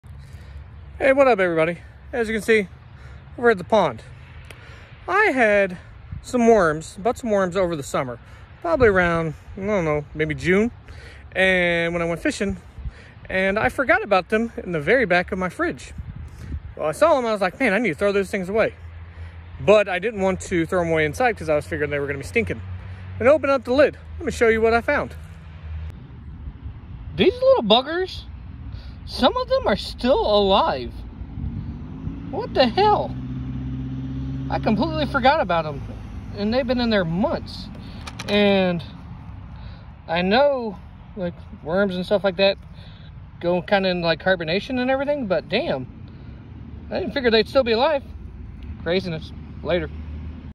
How Long Can Worms Survive In A Fridge
I forgot about these for months in the back of my spare fridge.  I can't believe they were still alive.  My pond fish loved them.

I would be honored if you would consider a membership to my channel.

Facebook.com/harshmanhills
Twitter.com/harshmanhills
Instagram.com/harshman_hills